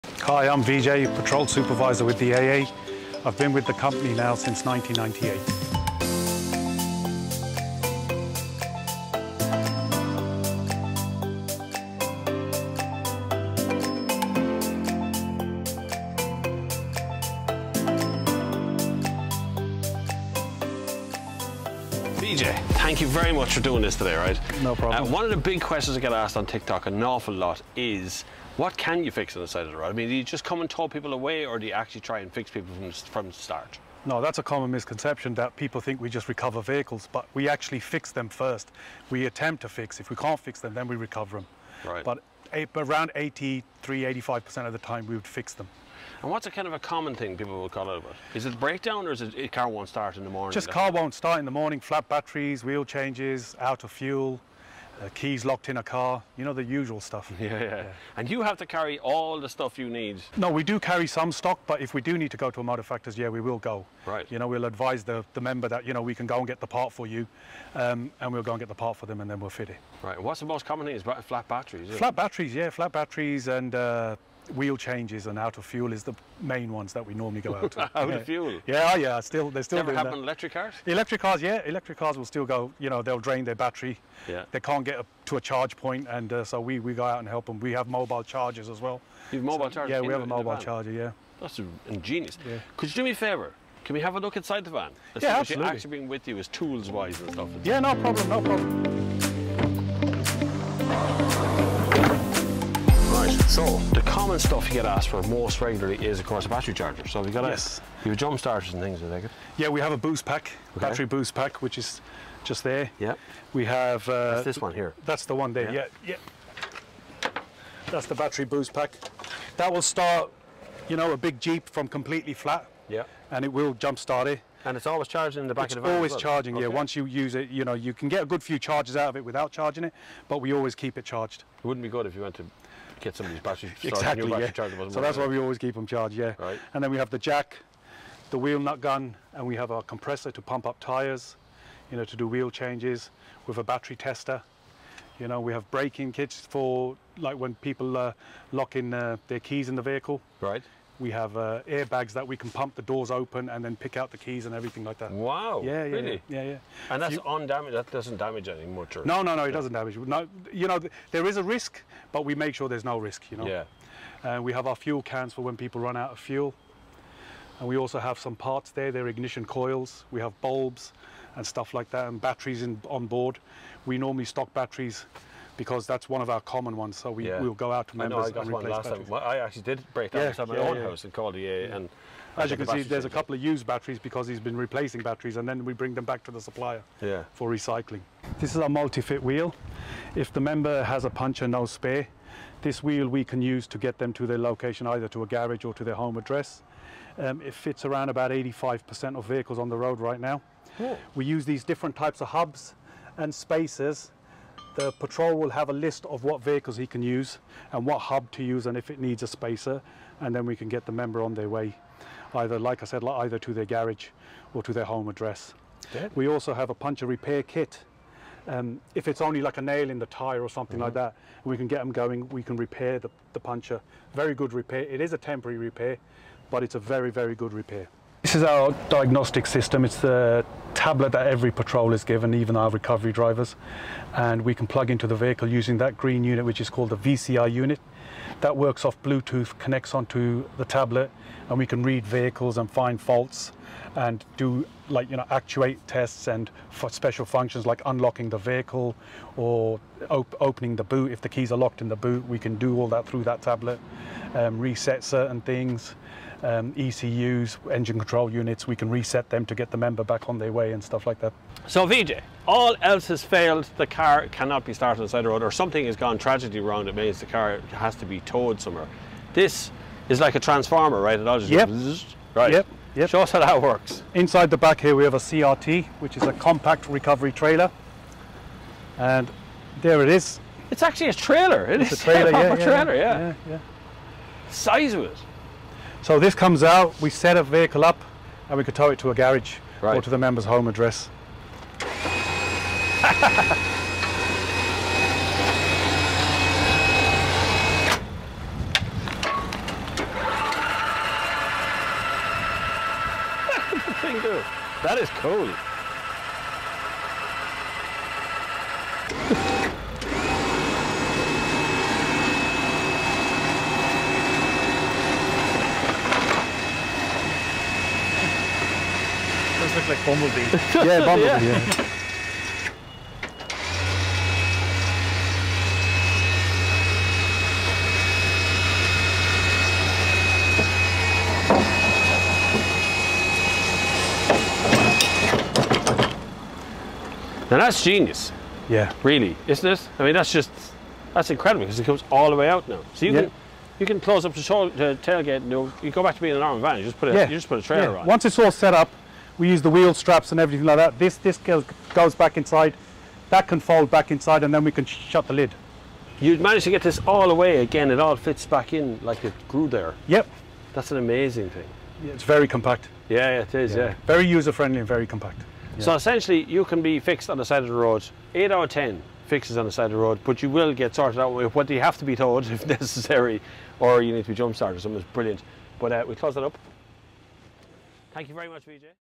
AA Roadside Rescue: What is in the back of an AA Patrol Vehicle
AA Roadside Rescue can repair 8 out of 10 vehicles at the side of the road? But how do they do it? @BobFlavinVideo speaks to AA Roadside Rescue Supervisor Vijay Gangarh about what is in the back of an AA Van.